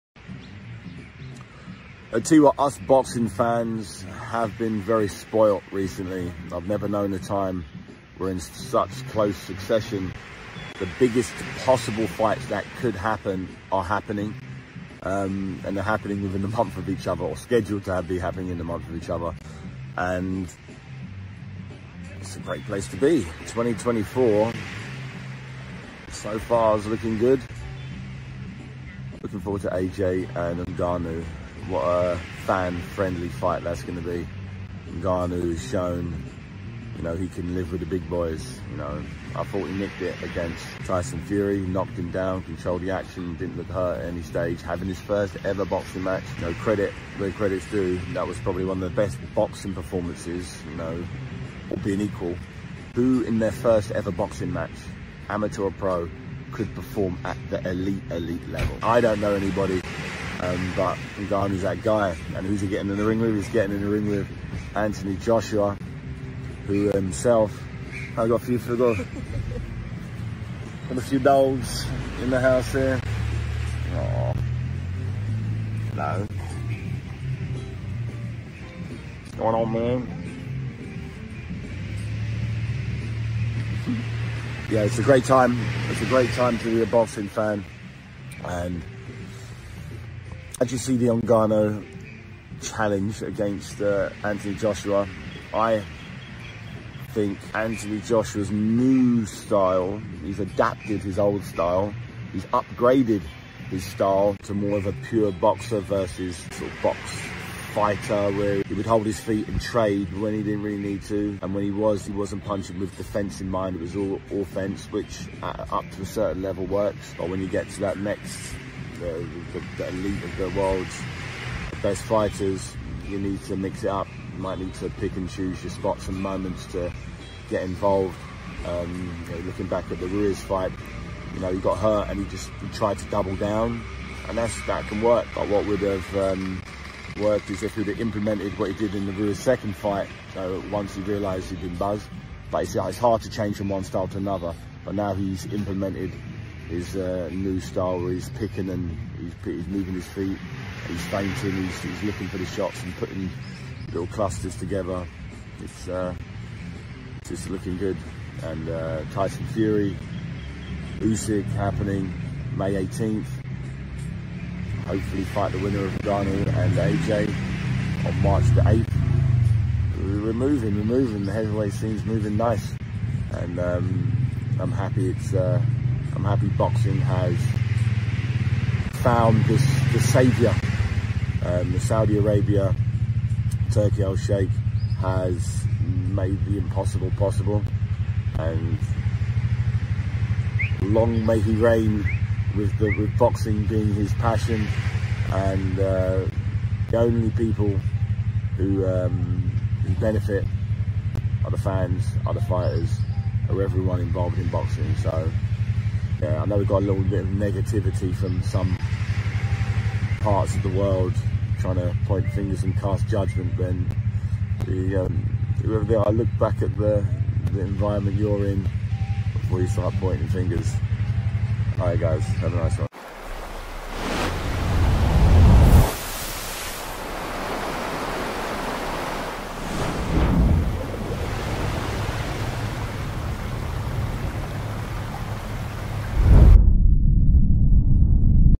Saudi have taken over !
Saudi have taken over and are transforming the boxing landscape.